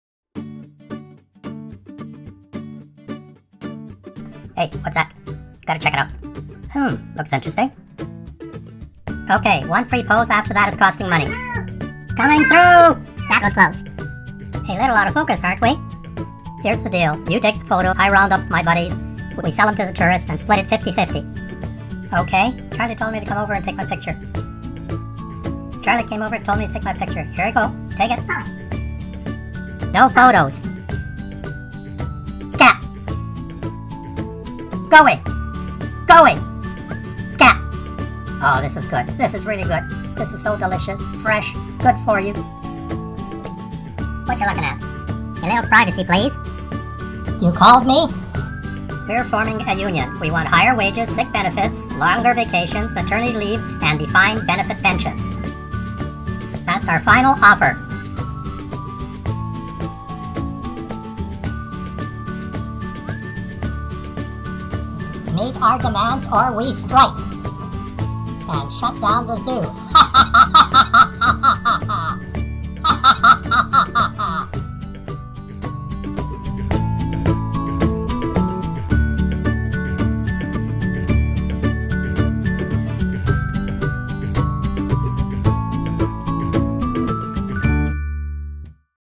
Assiniboine Park Zoo Gophers demand their Rights-2014
On "cheap" Tuesday I moseyed over to the Assiniboine Park Zoo.
If you haven't visited the zoo in a while you're in for a very pleasant surprize. massive changes! On my way out of the zoo, I came across a colony of curious gophers. My camera was of great interest to good number of these perpetual bundles of energy. Hope you enjoy my first attempt to do "voice overs" .6 hours to produce a minute and a half of video! Silly...but it was fun!